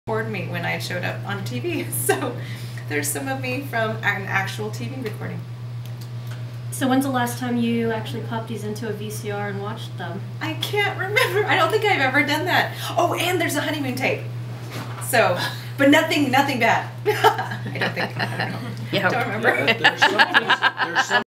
When was the last time you watched your home videos?
A short interview with Stephanie in the YesVideo offices, talking about how often she has watched her old videos.